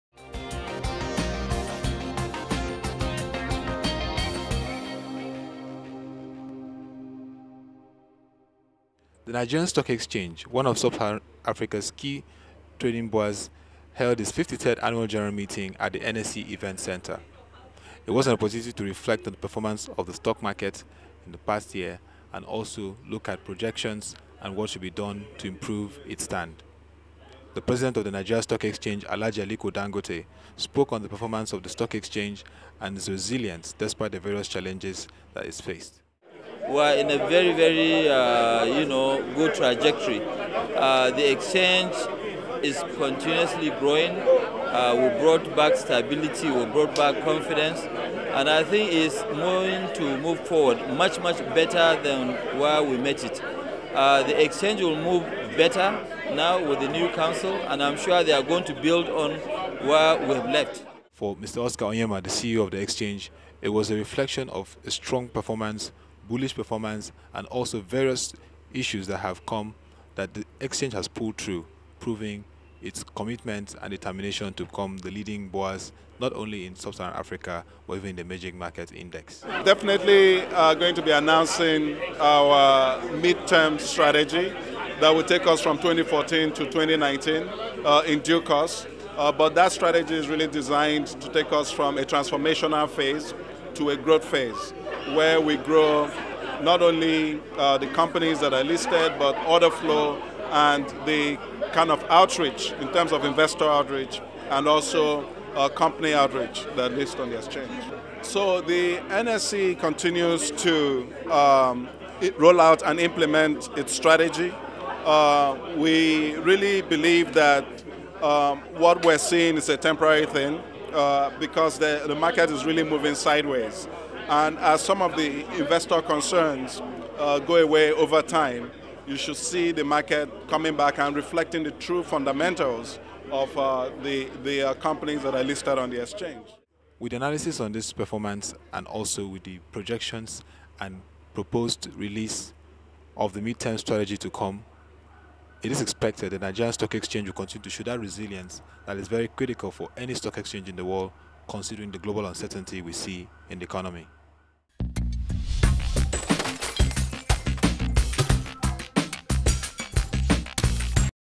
Highlights of the NSE 53RD AGM
Sub-Saharan Africa's leading investment hub, the Nigerian Stock Exchange held its 53rd Annual General Meeting at the NSE Event centre, Lagos.
Considering how the Nigerian capital market has evolved through the years of the market crisis era of 2007, to becoming one of the top ten key markets in the globe, the story of the  Nigerian stock exchange, will be that of 'Recovery to Stability'.
The President of the Nigerian Stock Exchange Alh. Aliko Dangote and the CEO Mr Oscar Onyema speak on the performance, trajectory, growth in the midst of global economic uncertainty and proposed mid-term strategy and goals of the exchange.